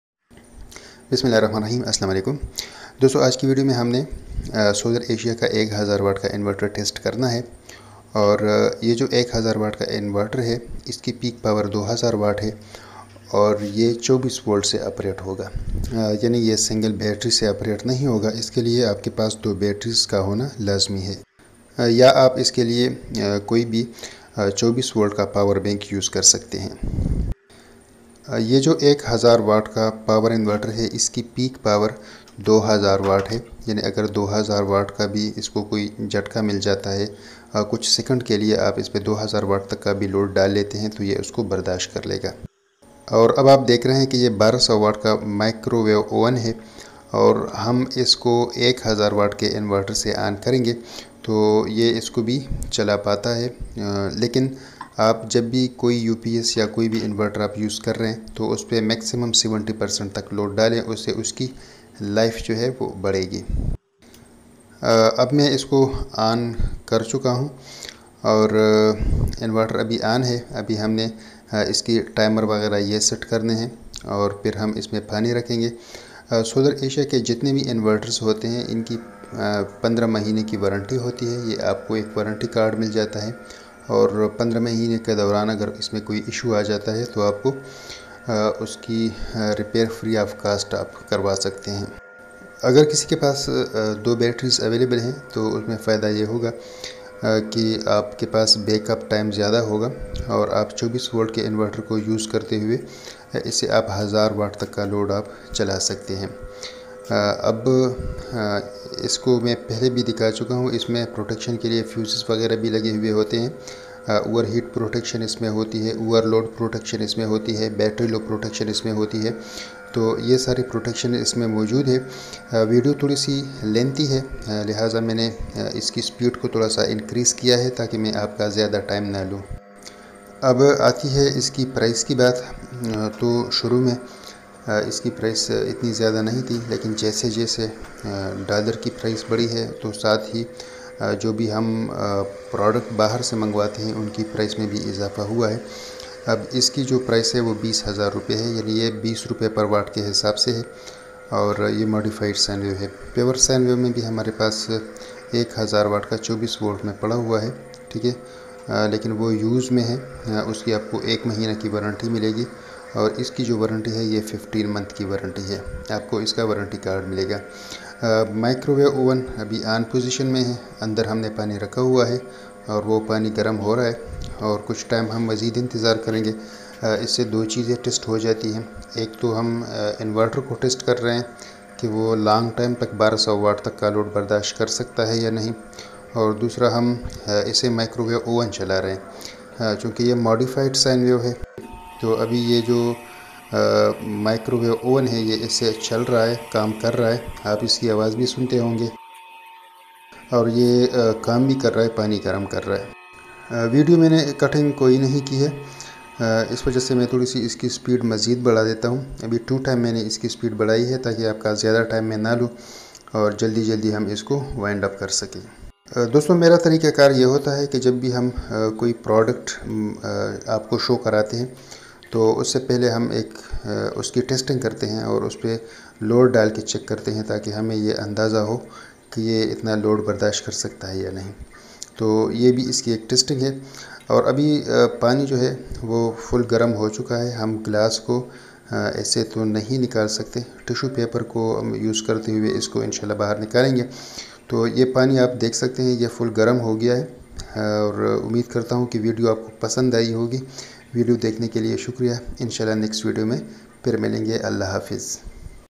1000watt Solar Asia Inverter,Load testing,15months Warrenty
1000watt Solar Asia Inverter,Load testing,15months Warrenty,

Aj ki video mai hum 1000 watt k inverter se 1200 watt ka micro wave owen chala k daikhengy.
24v 1000 watt k modified sine wave k ye inverter15 months warrenty ksath available hain.